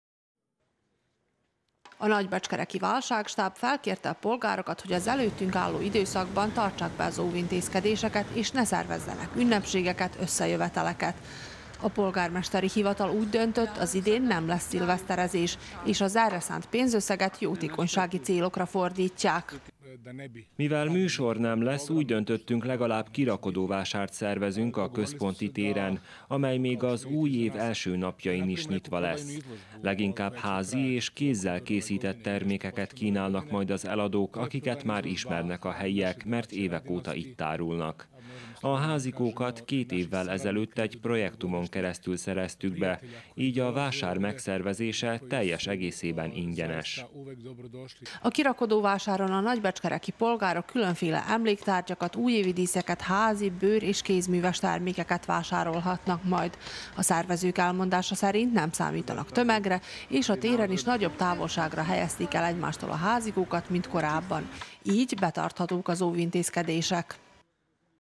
Szilveszteri program helyett jótékonykodás
A nagybecskereki önkormányzat azt a pénzösszeget, amelyet a városi költségvetésből szilveszteri programokra szánt volna, jótékonysági célokra fogja fordítani, így az idén a járványhelyzet miatt elmarad az ünnepi program.